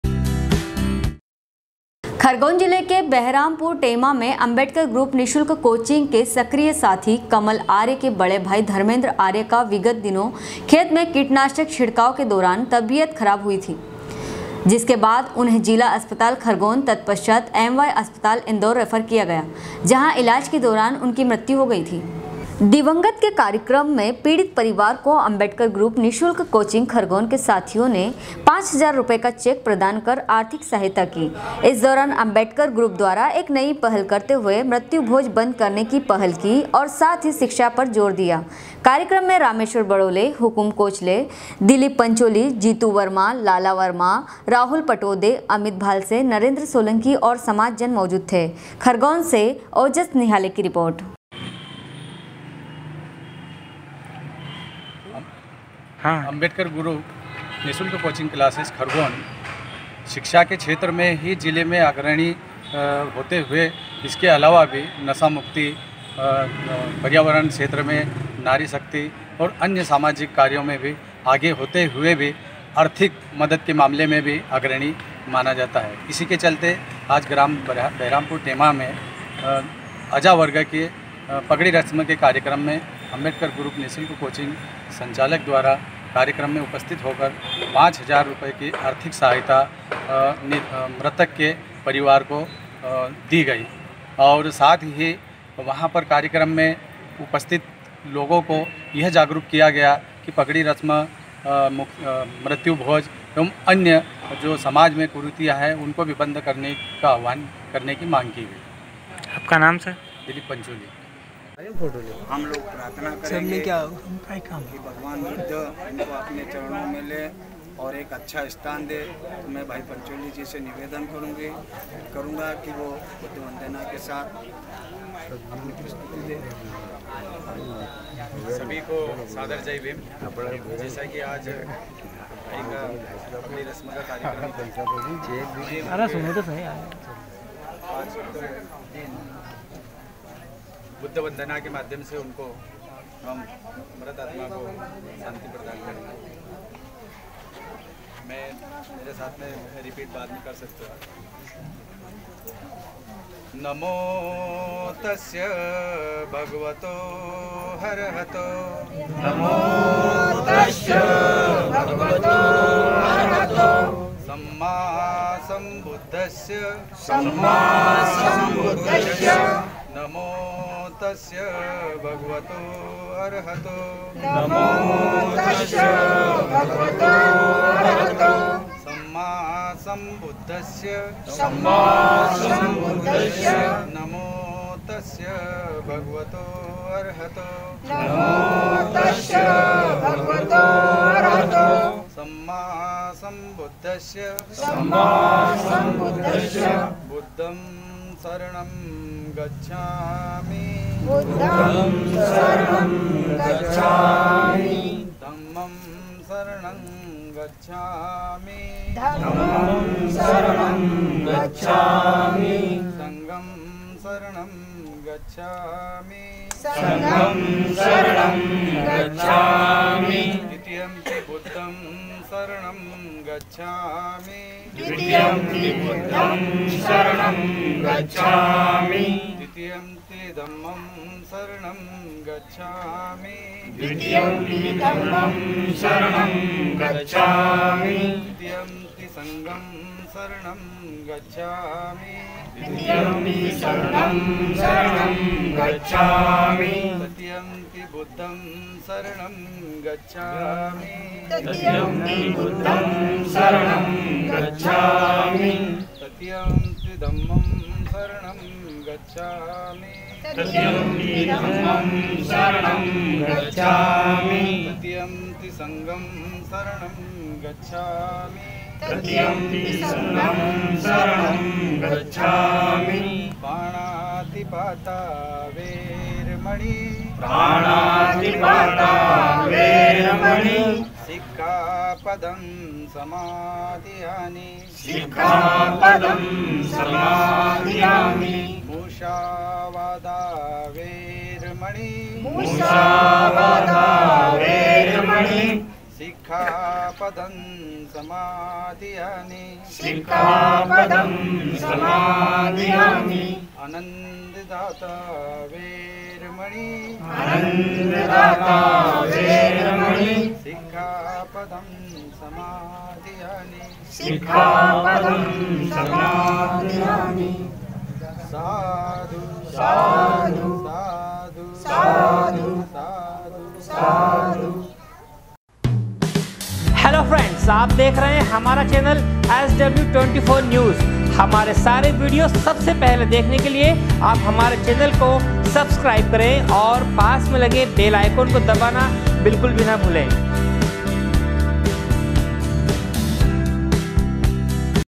SW 24 News Madhyapradesh
खरगोन जिले के बेहरामपुर टेमा मे अम्बेडकर ग्रुप निःशुल्क कोचिंग के सक्रिय साथी कमल आर्य के बड़े भाई धर्मेंद्र आर्य का विगत दिनों खेत में कीटनाशक छिड़काव के दौरान तबीयत खराब हुई थी जिसके बाद उन्हें जिला अस्पताल खरगोन ततपश्चात एम व्हाय अस्पताल इंदौर रेफर किया गया जहाँ इलाज के दौरान उनकी मृत्यु हो गई थी। खरगोन से ओजस निहाले की रिर्पोट